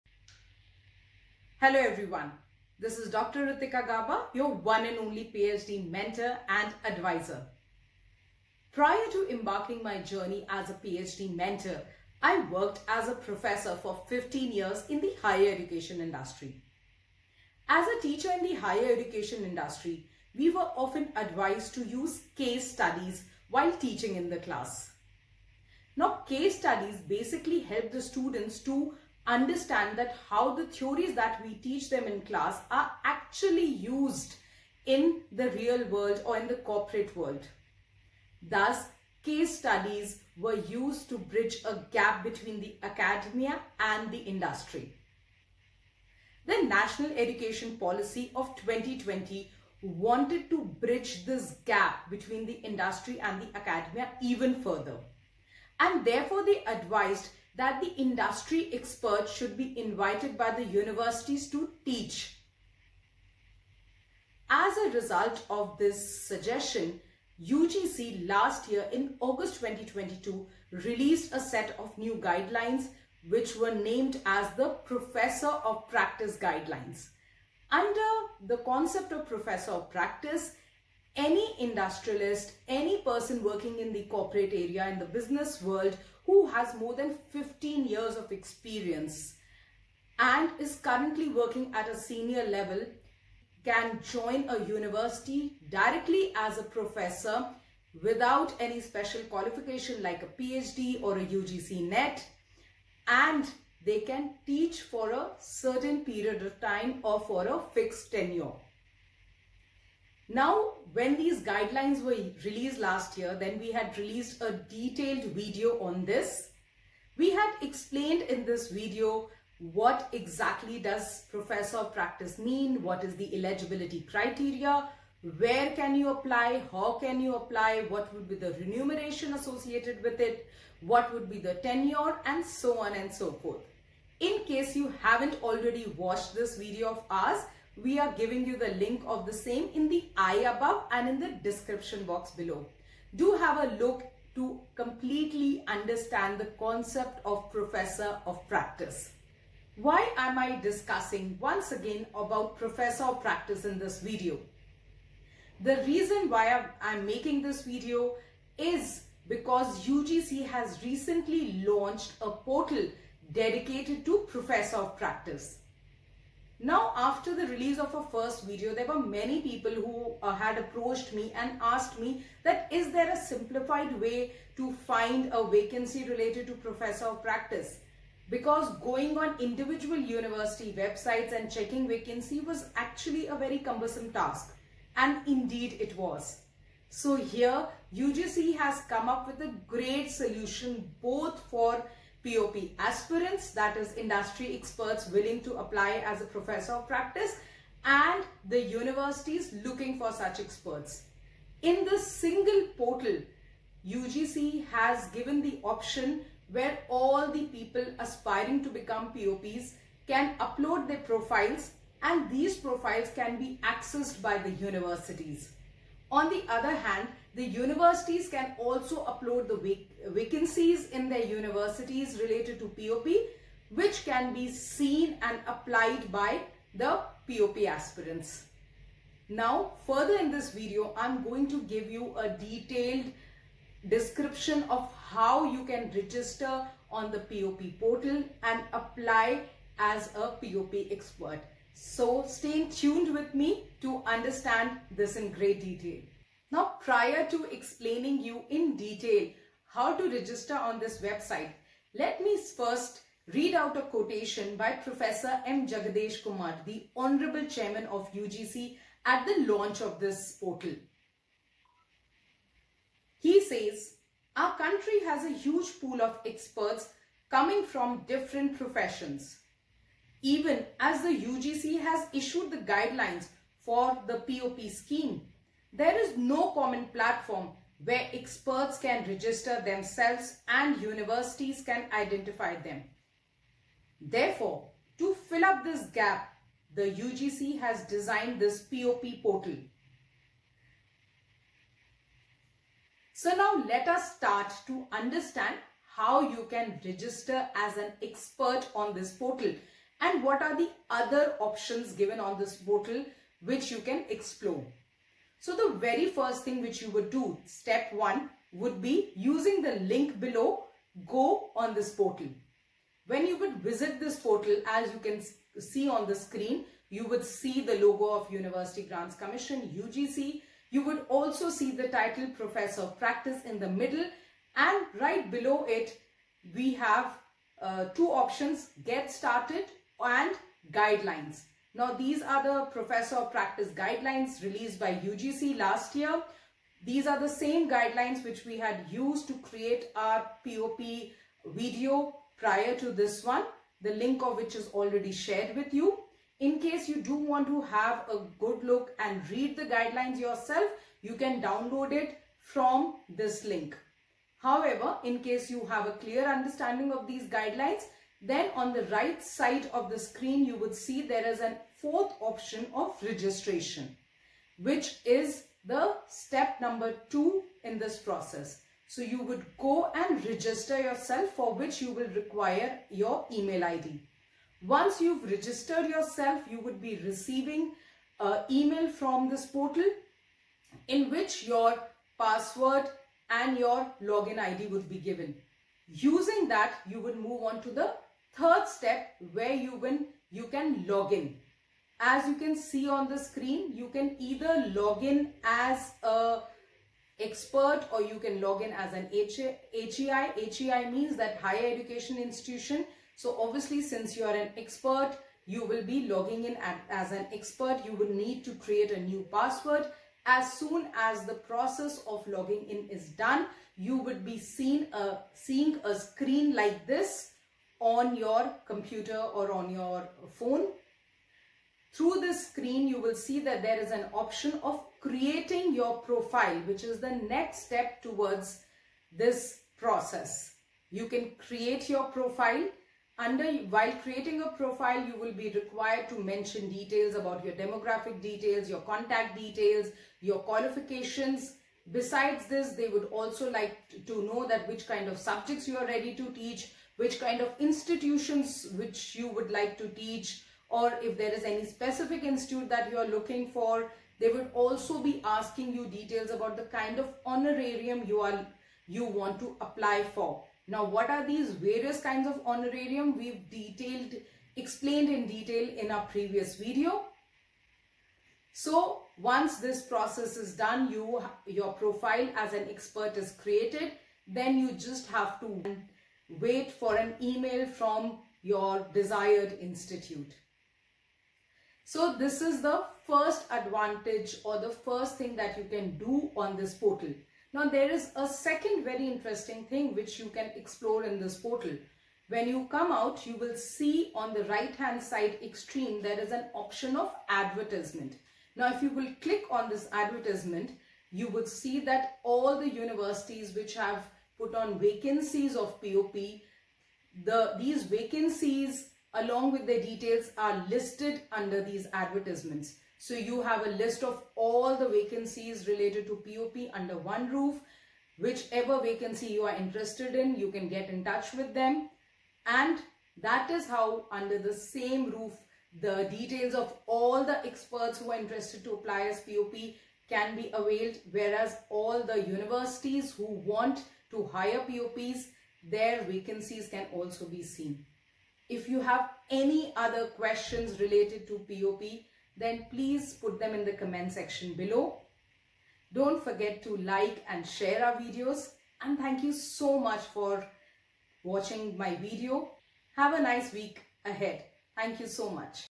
Professor of Practice / UGC PoP Portal | Professor without Ph.D. | PoP vacancies Portal
A few months back UGC released a guideline about Professors of Practice.

So basically in these guidelines, UGC had said that people from the industry or people with rich corporate experience. With at least 15 years of experience can apply, without any PhD or Special qualification for the post of Professors in the higher education industry.

We have already made a complete video on this topic where we have told you in detail

1. Who is a PoP
2. The eligibility for PoP
3. Where can you apply & How
4.. What will be the remuneration
5. What is the tenure of PoP

Professionals found it difficult to find suitable vacancies for PoP positions . Similarly even for universities  finding suitable candidates was a challenge.

Thus a few months back UGC , to ease the process of PoP, created a pop portal .

On this portal all PoP Aspirants can create their profile and universities can display the vacancies for PoP.

In this video, Dr. Ritika Gauba has explained in detail the process of PoP application through this portal.

Step-by-step guidance about registration, login process, profile creation, and finding suitable vacancies is given in the video.

So if you are also keen to understand the PoP process, watch the video.

In case you want to learn more about PoP then watch our previous video dedicated to this topic by clicking on the link 👇